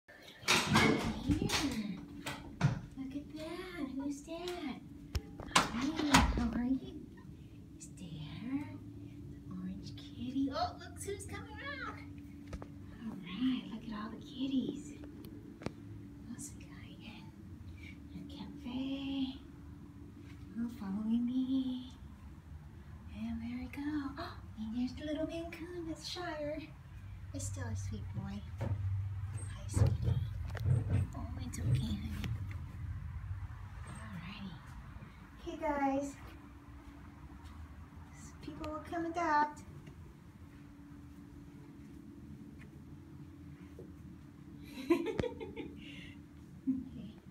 Catfe Cats 2-27-19 (ADOPTED 3/7/19)
Come meet our cats in Sacramento area's first Catfe. These cats arrived to the shelter after their owner was involved in a car accident and was hospitalized. Most of the cats are outgoing, affectionate, and loving, and will probably greet you at the door when you visit them. The black kitten is particularly playful. There is also a shyer Maine Coon who will probably be hiding, but still a sweet boy. They would love to be adopted with at least one other sibling. Two for one pricing applies if you do!